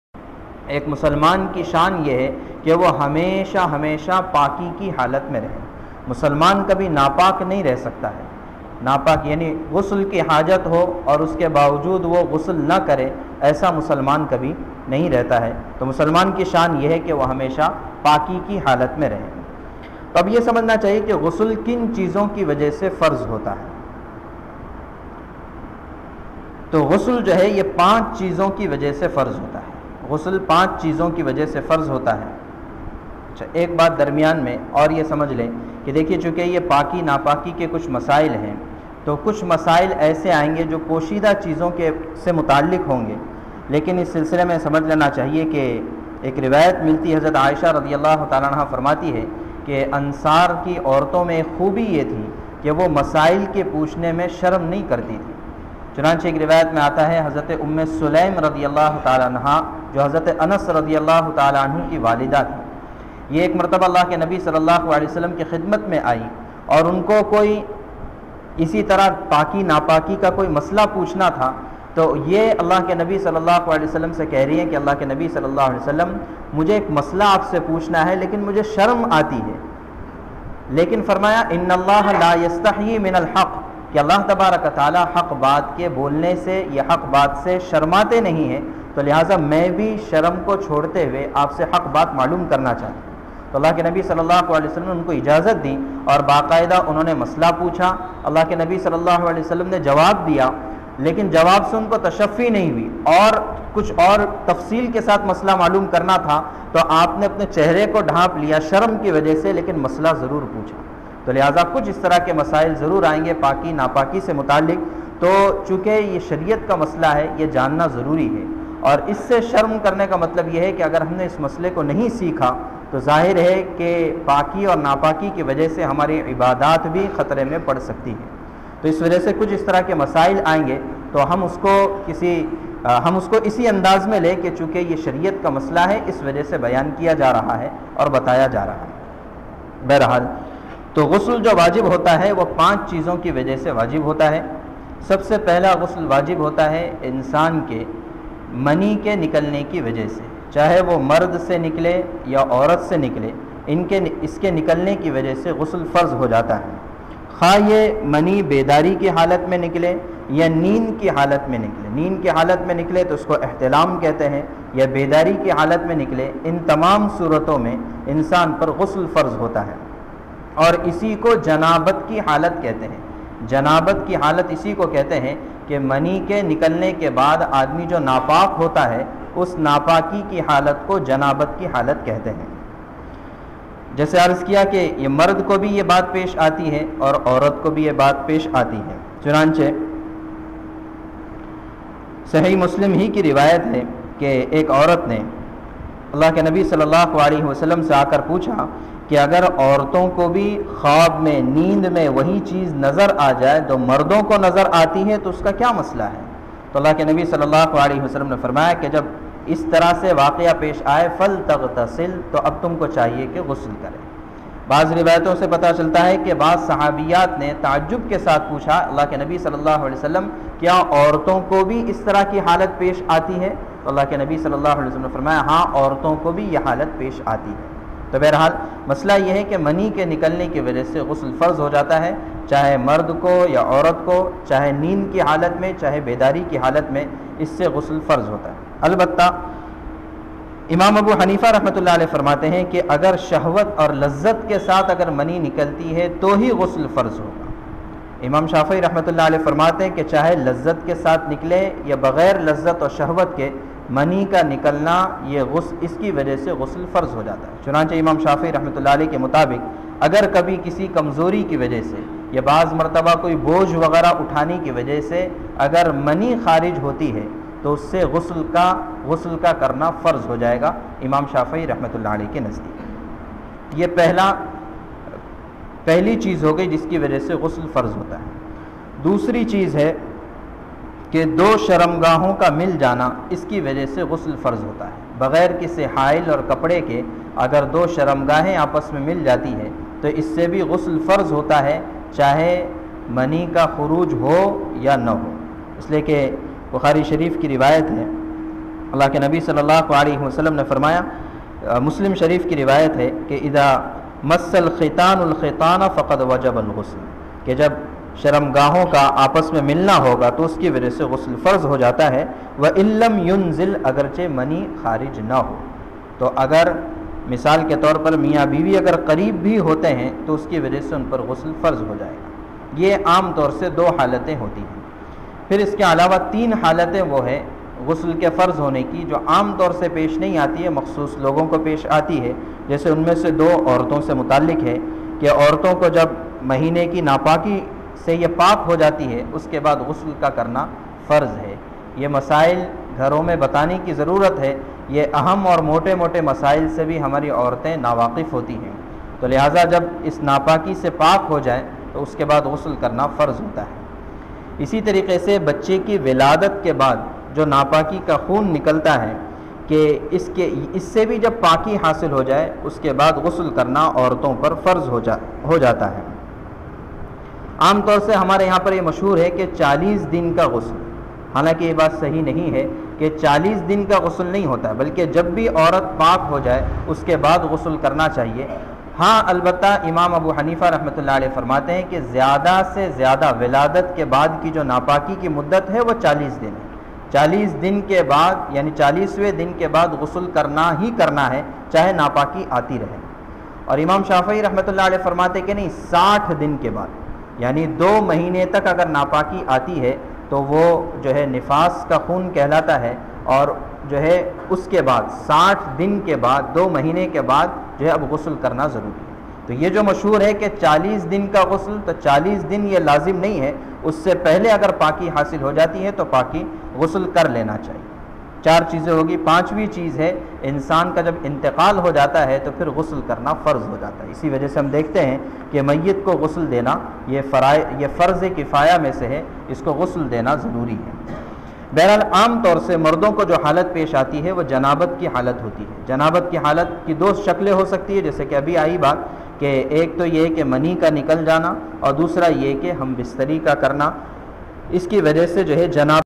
3 Gusl kin chizon se Farz hota hai
Series: Masa'il e Gushl (Bathing)
Speaker: Mufti Taufeeq Mansoor Mazahiri
Student of Shaikh Yunus Jaunpuri (Rh)| Rector Madrasa-tus-Suffa, | G. Secretary JamiatUlama Ratnagiri, | Member @Rehbar_Fin Shariah Board